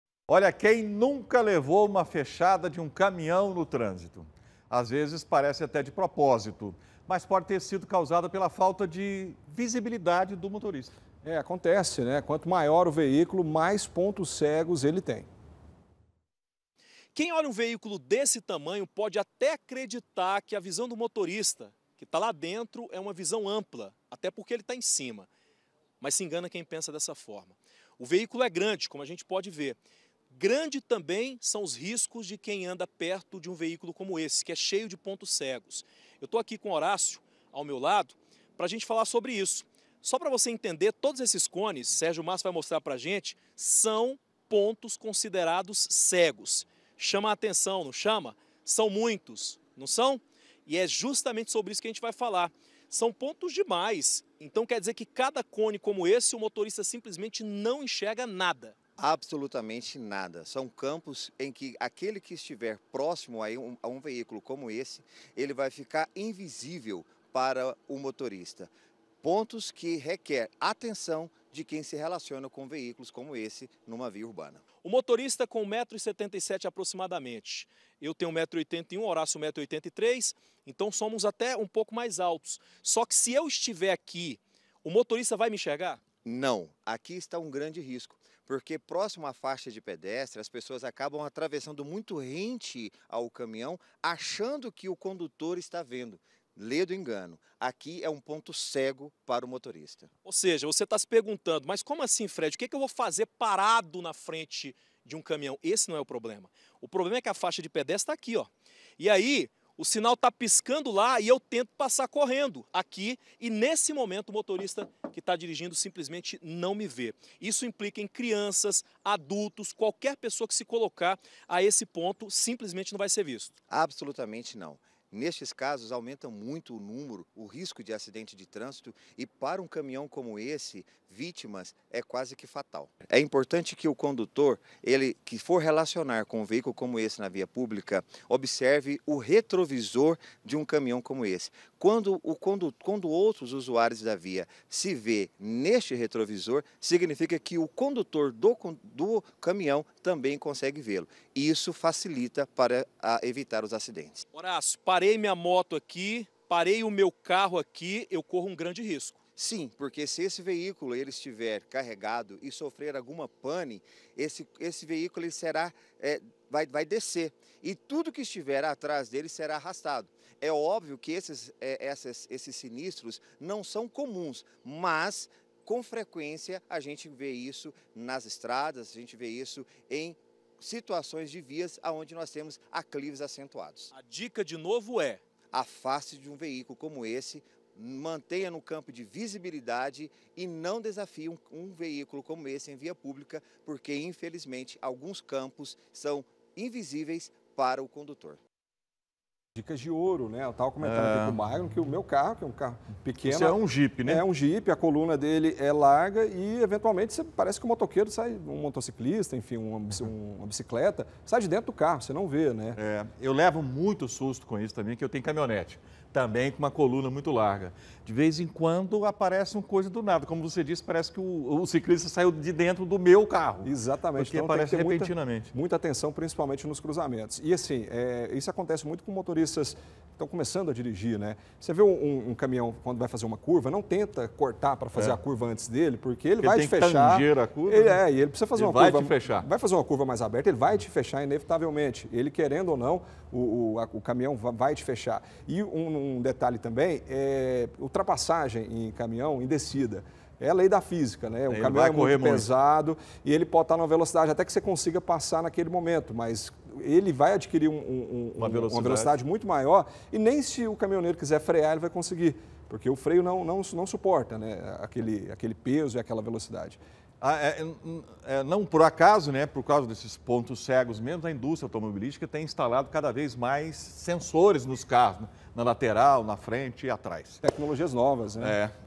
PONTOS CEGOS CAMINHÕES
PONTOS CEGOS - FALTA DE VISIBILIDADE PODE PROVOCAR ACIDENTES
QUEM NUNCA LEVOU UMA FECHADA DE UM CAMINHÃO NO TRÂNSITO?  ÀS VEZES PARECE ATÉ PROPOSITAL, MAS PODE TER SIDO CAUSADA PELA FALTA DE VISIBILIDADE DO MOTORISTA. QUANTO MAIOR O VEÍCULO, MAIS PONTOS CEGOS ELE TEM.